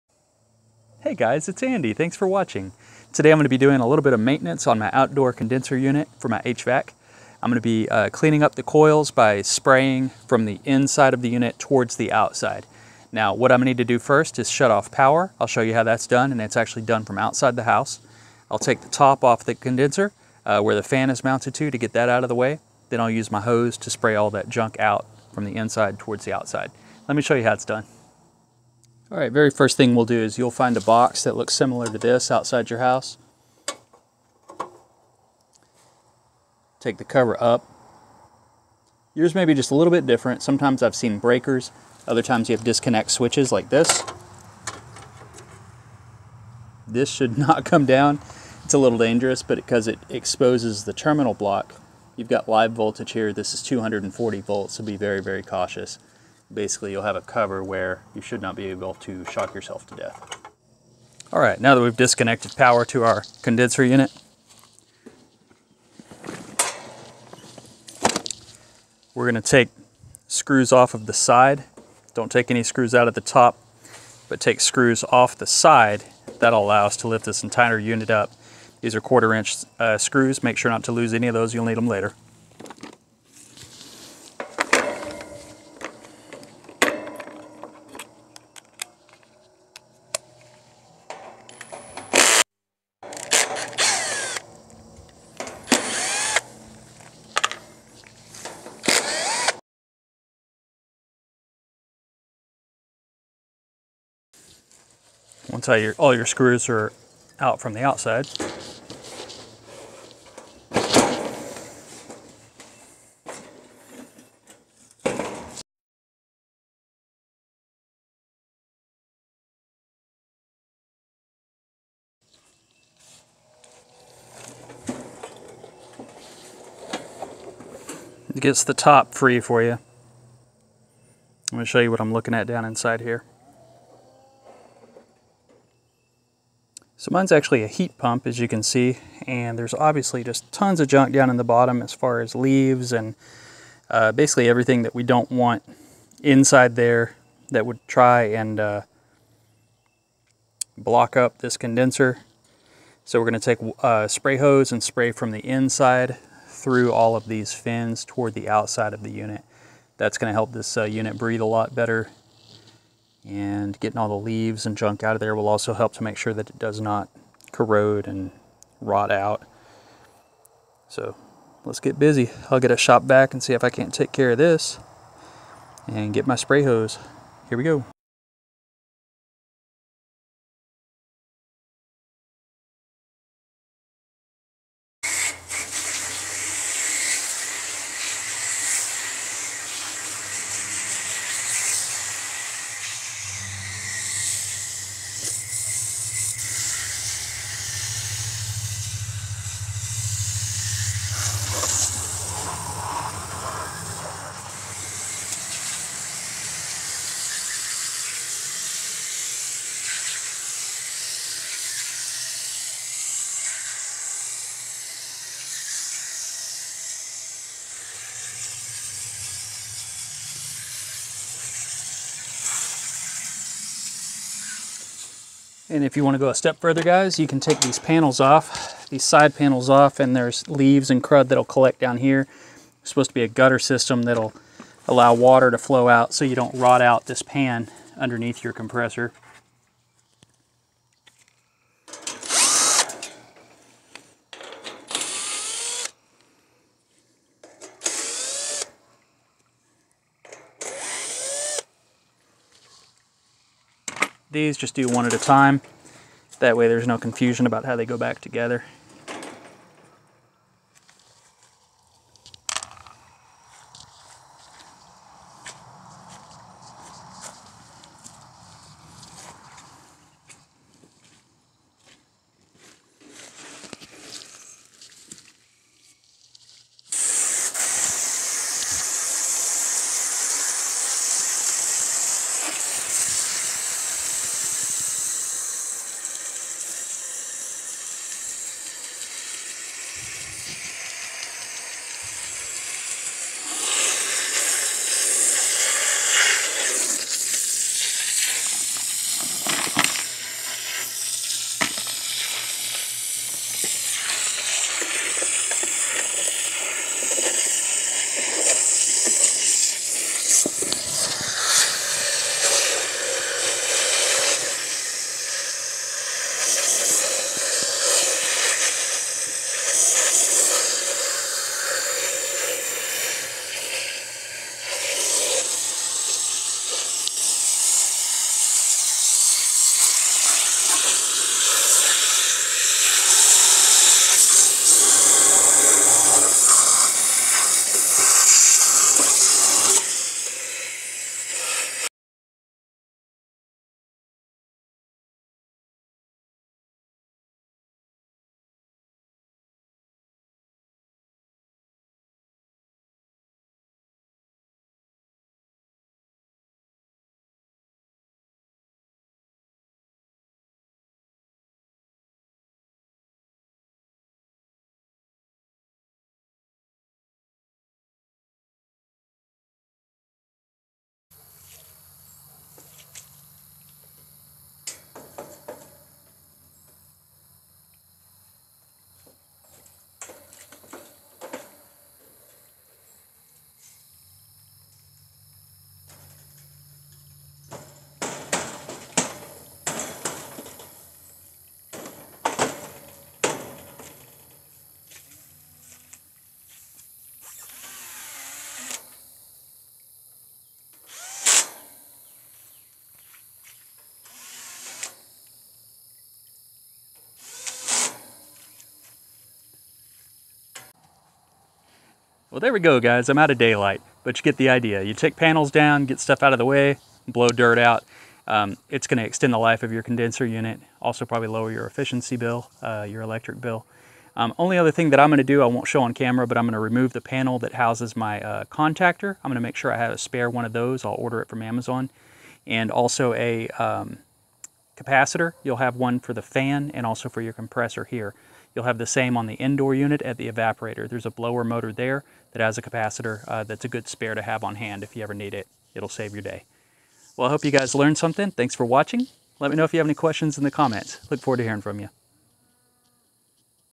Cleaning HVAC Condenser
Cleaning my outdoor condenser unit for my HVAC air-conditioner.
Doing this maintenance as part of your annual maintenance will help your unit last longer.

What is a condenser anyway?
A condenser is a component of a larger sealed system, used in both refrigeration or HVAC. Cooling is achieved by collecting heat from one environment and transferring it to an environment where the heat is non-objectionable (i.e. outside for example). The condenser is where this exchange happens.  Heat is transferred to a cooler evaporator (inside) and carried by refrigerant (typically) to outside your home where the heat can be transferred to the outside environment.

So, keeping this part clean and free of dust and leaves makes this heat transfer much more efficient. It helps also with making sure your system does not need to run as long to achieve cooling.